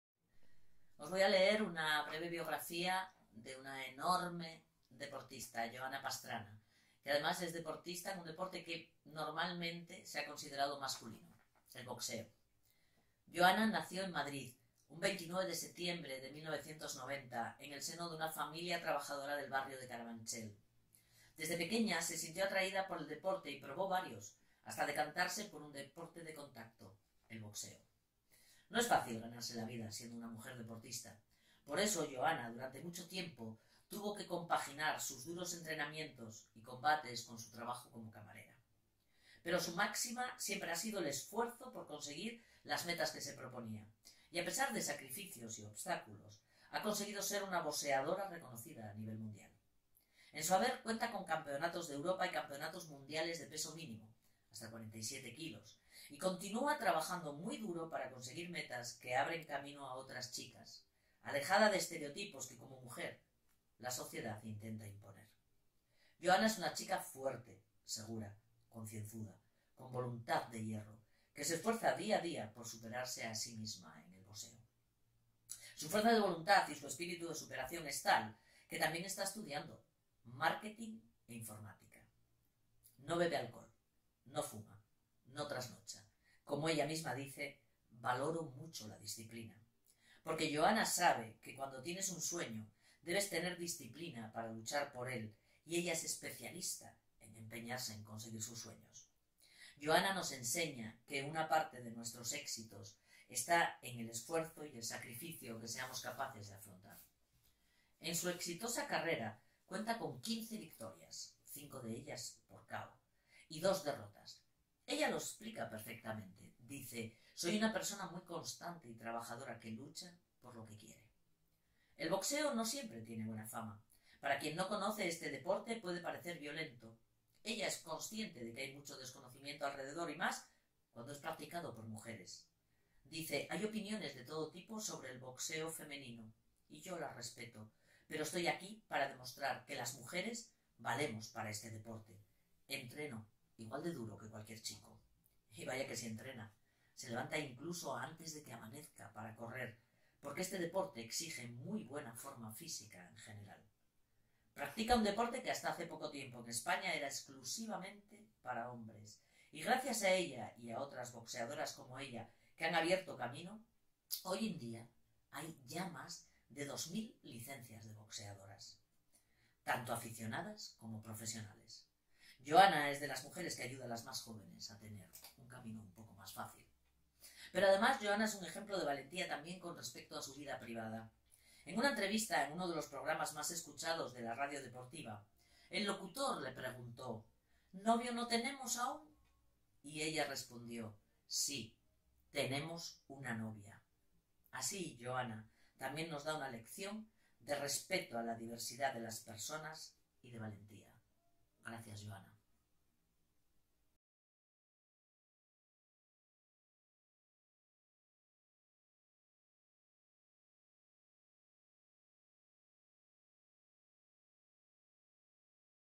Marian Moreno maratón de lectura
Marian Moreno Llaneza feminista de nacimiento, profesora y escritora española experta en formación de profesorado en coeducación.1Nacida en Asturias y licenciada en Filología Española por la Universidad de Oviedo presto su pluma para escribir tres de las historias del nuestras leyendas del Deporte en la publicación "De Niñas a Leyendas. 25 mujeres deportistas que han hecho historia", y hoy nos ha prestado su voz para leérnosla. Otra gran mujer que se suma al nuestro maratón literario DeNiñasALeyendas con la lectura de la pequeña biografía de Joana Pastrana. Esta actividad se enmarca dentro de la propuesta de Taller de Solidaridad YoMeQuedoEnCasa y EntrenoConciencia a través de la cuál ofrecemos distantas actividades para trabajar los valores en familia en tiempos del

Puedes comprar el libro impreso en nuestra tienda o descargarlo de forma gratuita en pdf en este enlace o /